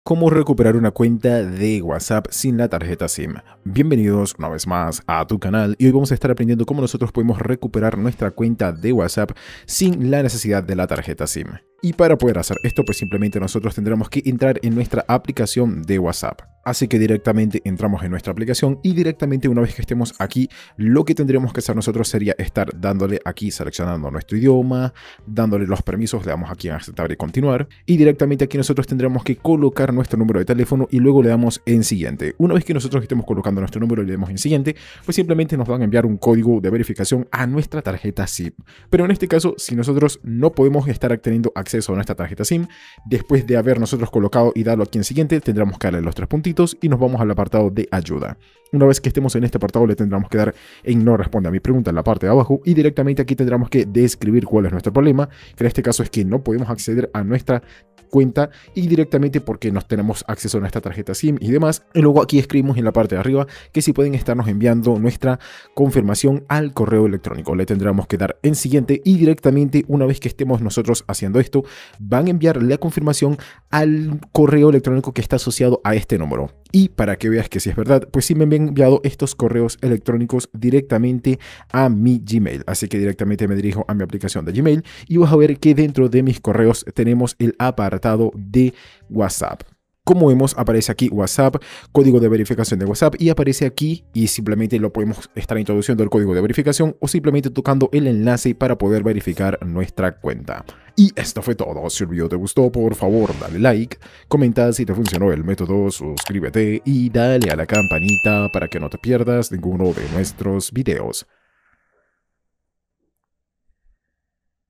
✅COMO RECUPERAR CUENTA DE WHATSAPP SIN TARJETA SIM
Aprende a como recuperar cuenta de WhatsApp sin tarjeta sim con este tutorial muy fácilmente.

00:00 Intro
00:10 Explicación